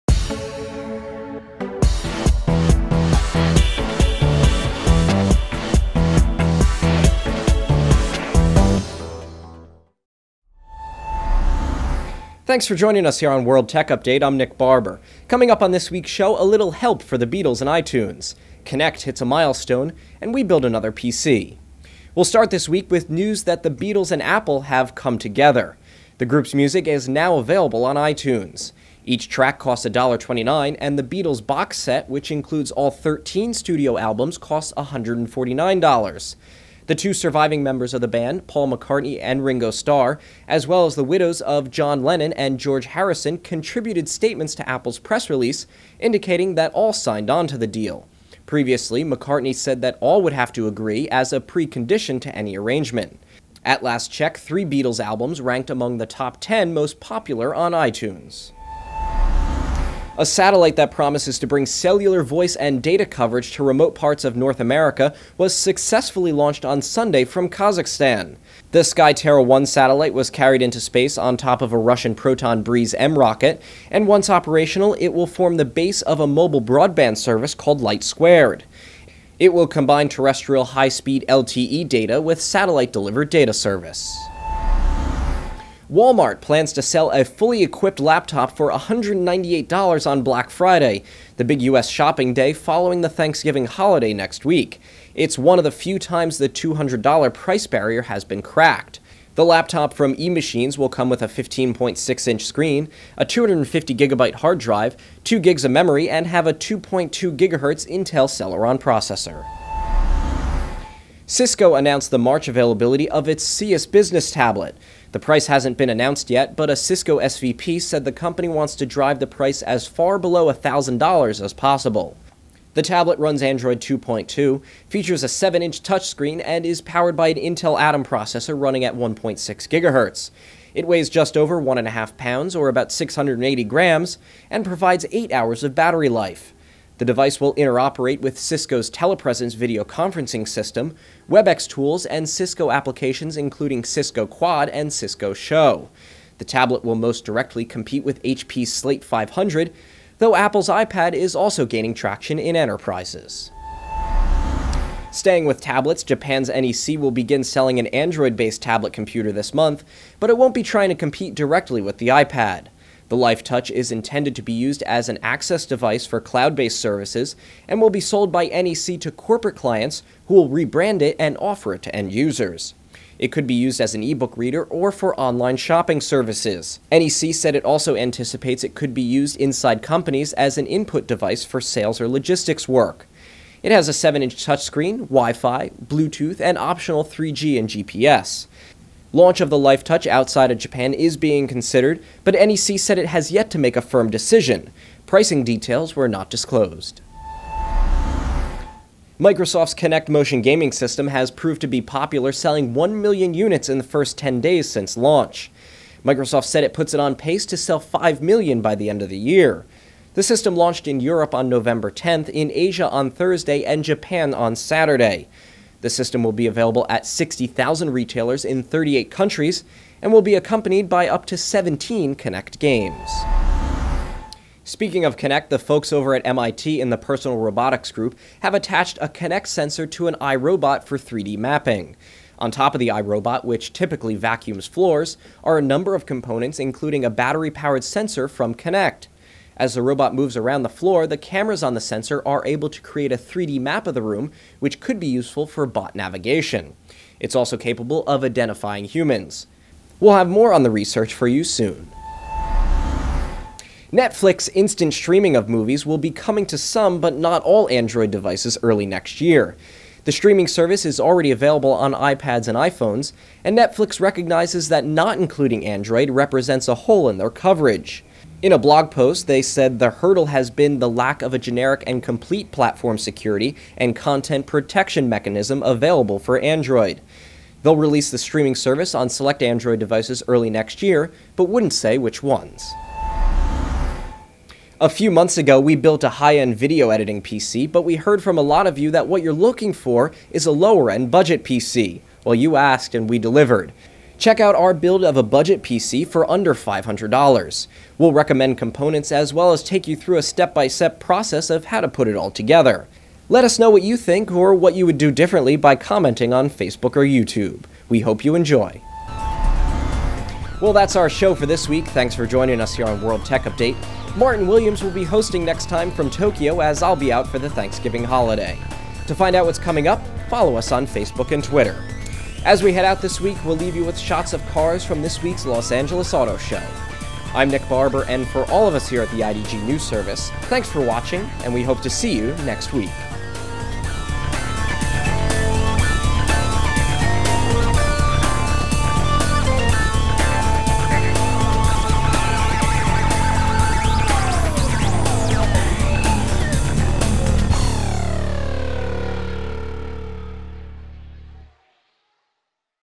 World Tech Update, Nov. 18, 2010
On World Tech Update this week the Beatles and Apple finally come together, a voice and data satellite launches, Walmart sells a cheap laptop, Cisco's Cius tablet gets a launch date, NEC shows of the LifeTouch, Kinect hits a milestone, Netflix plans to stream to Android and we build another PC.
Find out what's coming up on each week's show by following us on Twitter (WorldTechUpdate) or check out our Facebook fan